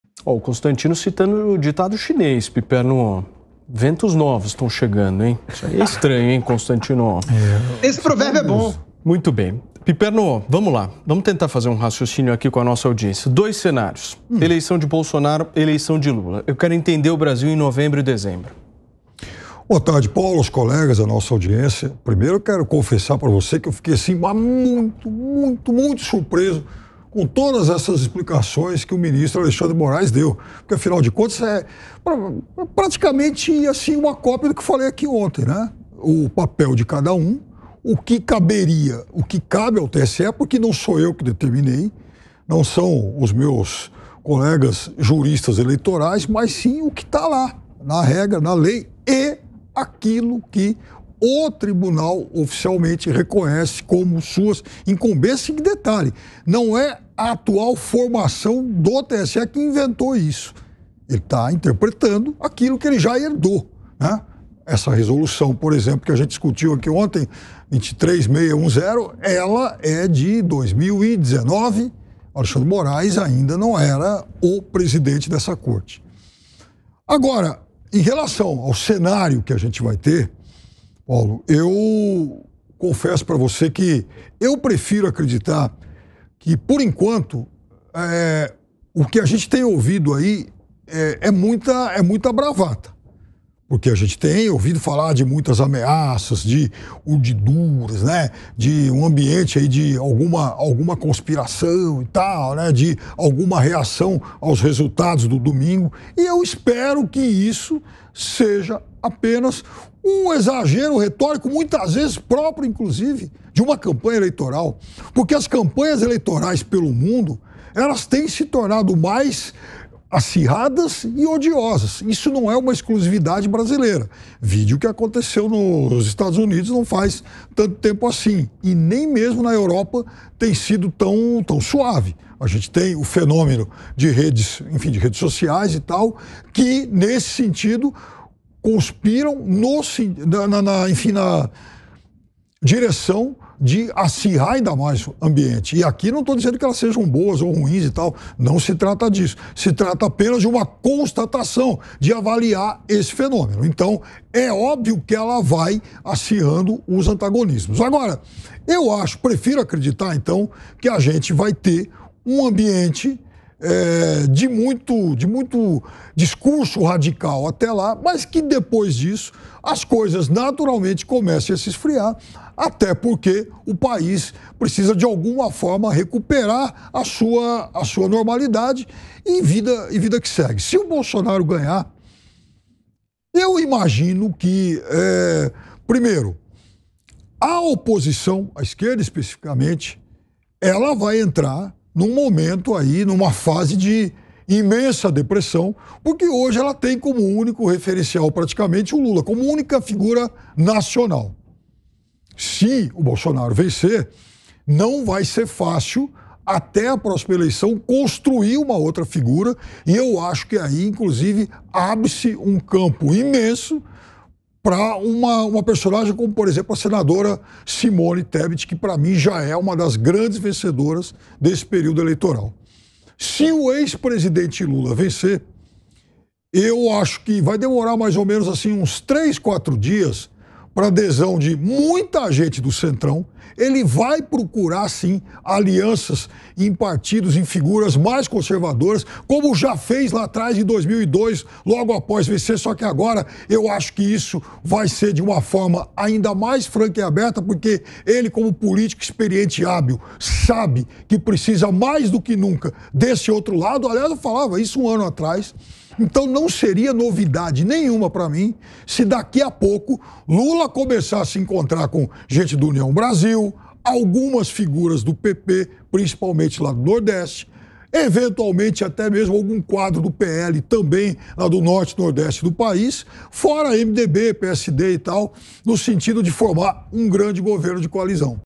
Fábio Piperno: Se Lula vencer as eleições, deverá buscar alianças com partidos mais conservadores
Fábio Piperno analisa as expectativas para o cenário eleitoral de domingo, com uma provável exaltação dos discursos especialmente pelas redes sociais, avaliando a carência de lideranças da esquerda caso Bolsonaro seja reeleito, e as mobilizações para alianças conservadoras em caso de vitória de Lula.